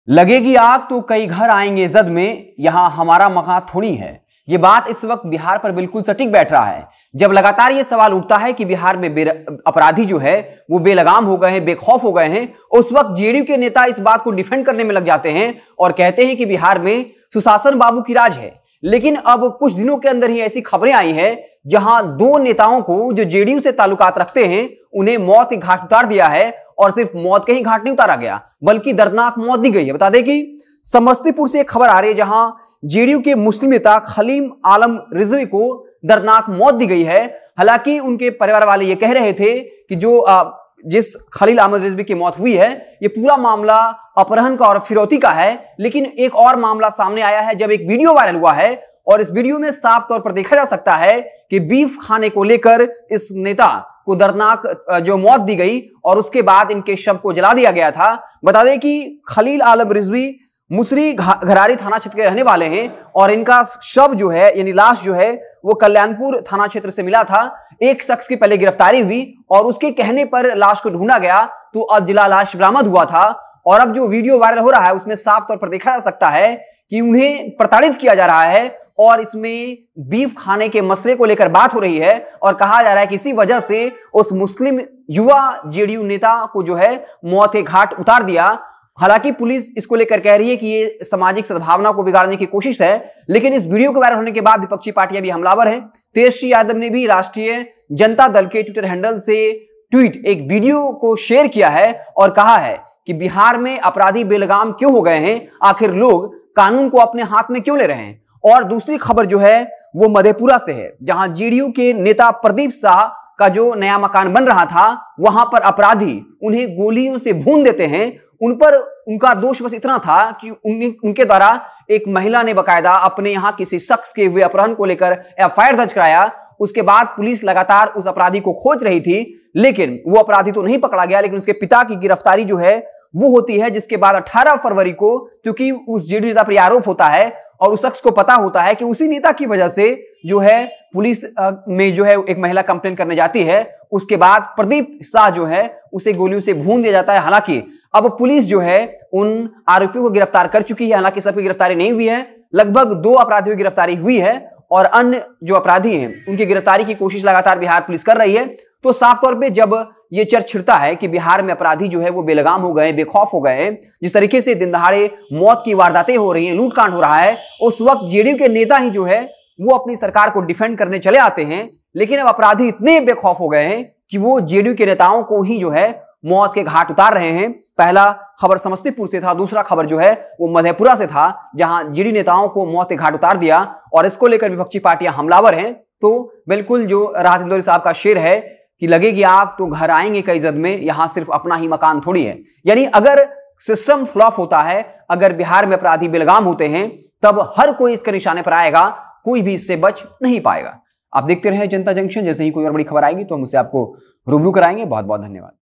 CM Nitish के राज में उनके नेता ही सुरक्षित नहीं है, दो नेताओं को मौत के घाट उतार दिया!
ये कैसा सुशासन राज है?जेडीयू नेता खुद सुरक्षित नहीं है!अपराधियों ने कुछ दिनों के ही अंतराल पर दो नेताओं को स्वर्ग भेज दिया!